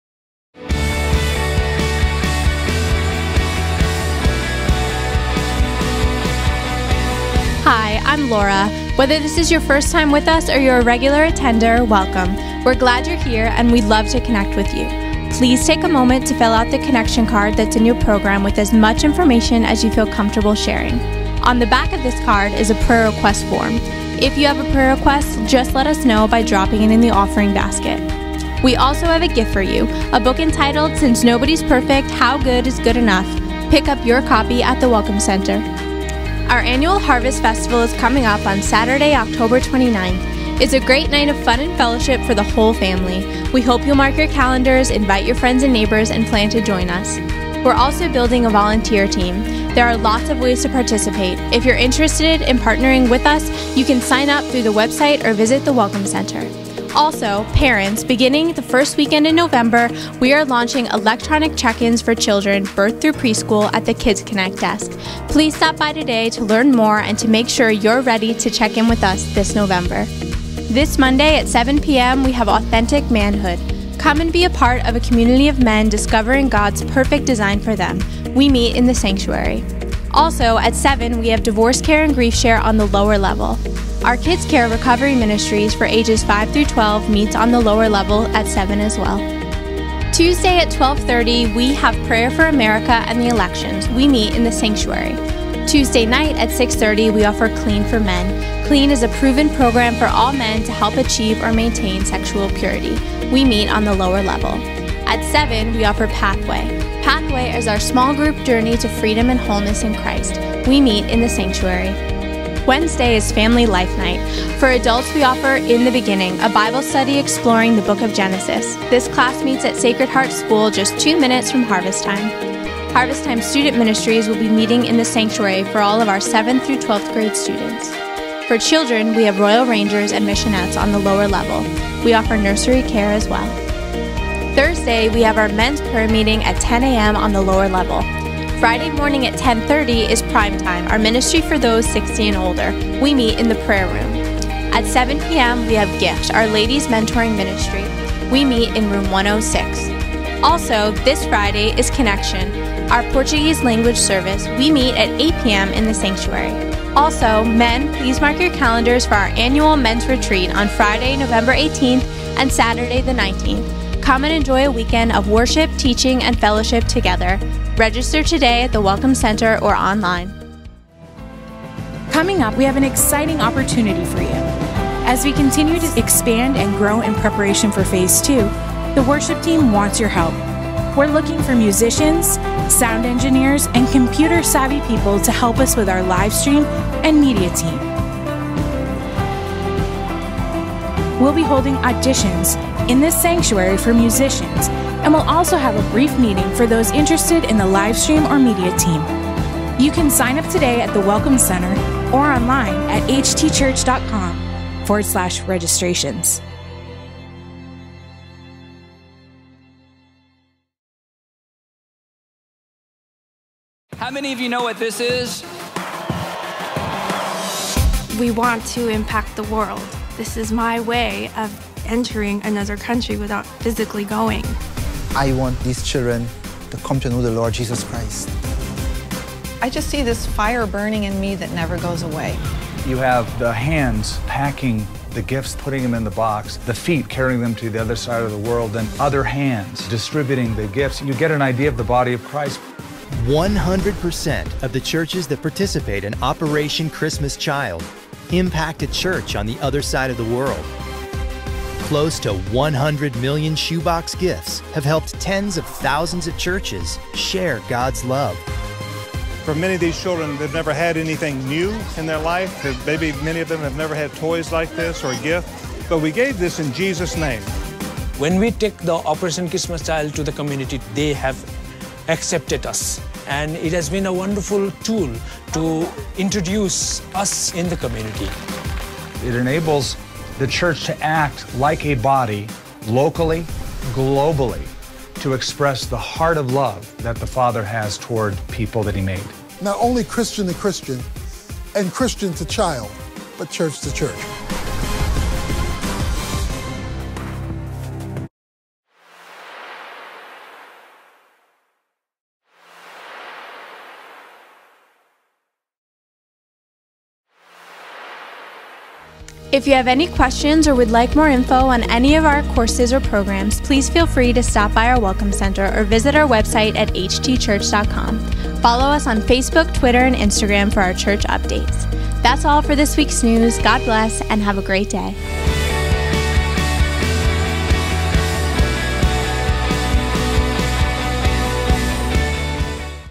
Harvest Time News (October 23, 2016)
News and upcoming events from Harvest Time Church, Greenwich, Connecticut